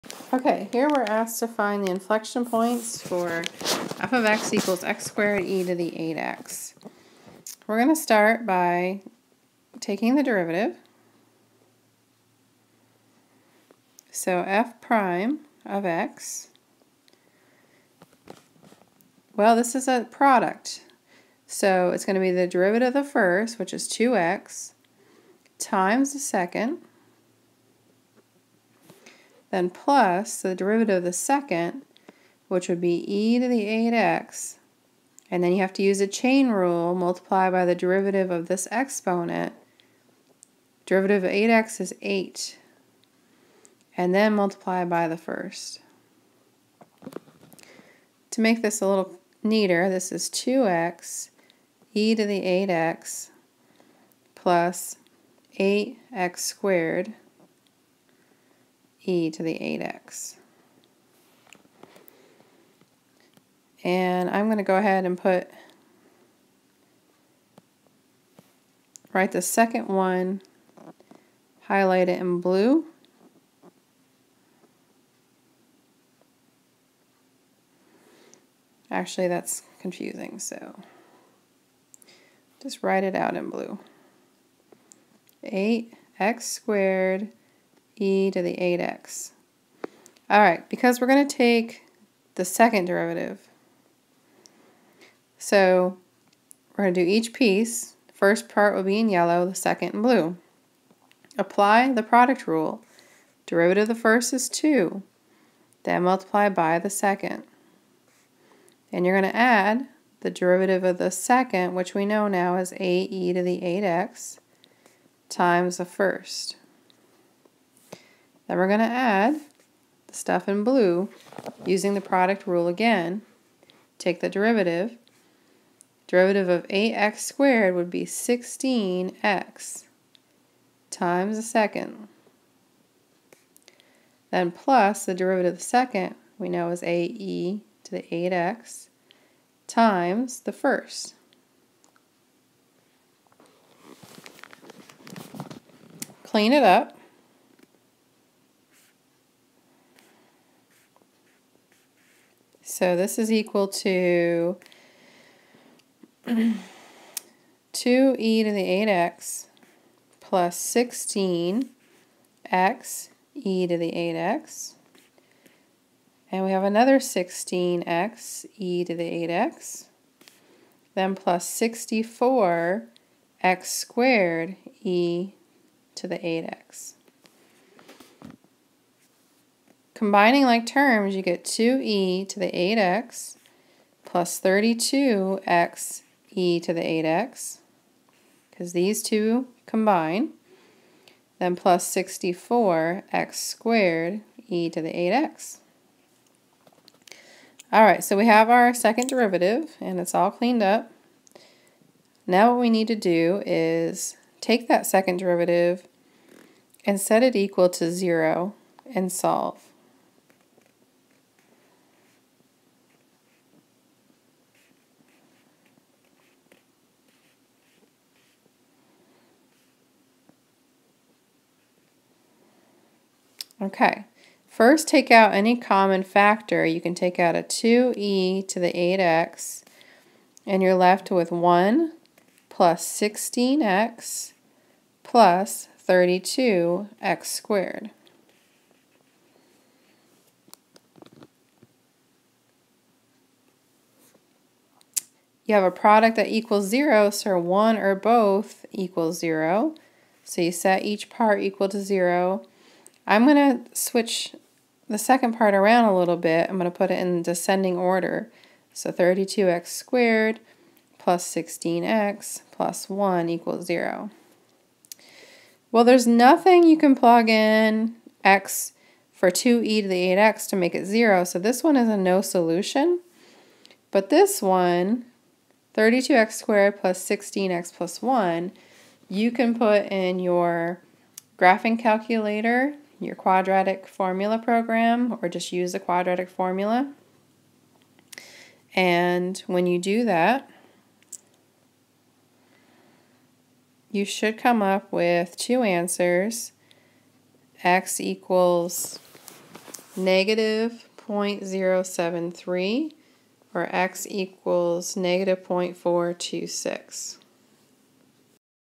MTH 270 2.5#8 - Finding Inflection Point
This video explains how to take a function which is a product and find the 1st and 2nd derivatives, then set the 2nd derivative equal to 0 to find the points of inflection.